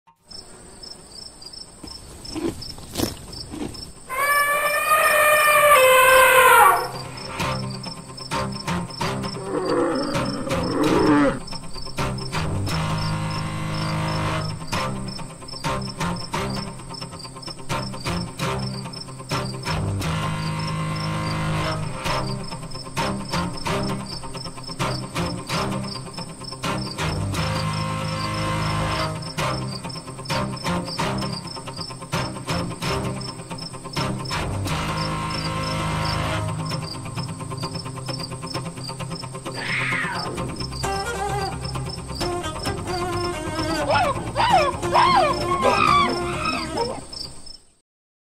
JUNGLE animals | Toy Animal Figurines Schleich, Papo, Mojö fun | Stop motion video
Schleich, Mojo fun, Papo animals from the jungle. Bear, elephant, rhinoceros, clouded leopard, orangutan. Many different animals live in the jungle.